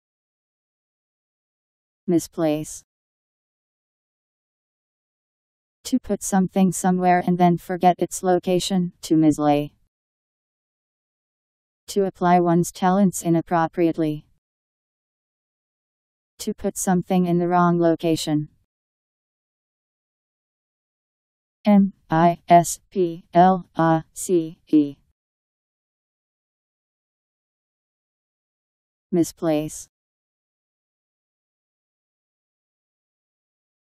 Misplace Meaning
Video shows what misplace means. To put something somewhere and then forget its location; to mislay. To apply one's talents inappropriately.. To put something in the wrong location..  Misplace Meaning. How to pronounce, definition audio dictionary. How to say misplace. Powered by MaryTTS, Wiktionary